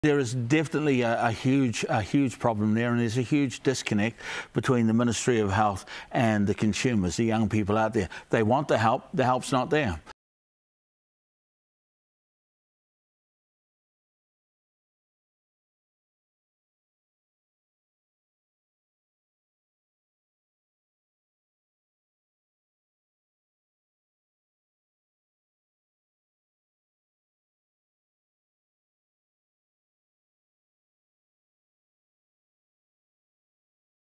Mike King calls out for more action on mental health
Founder of I Am Hope, mental health advocate Mike King says action is needed on mental health services as there has been no improvement for youth services in years.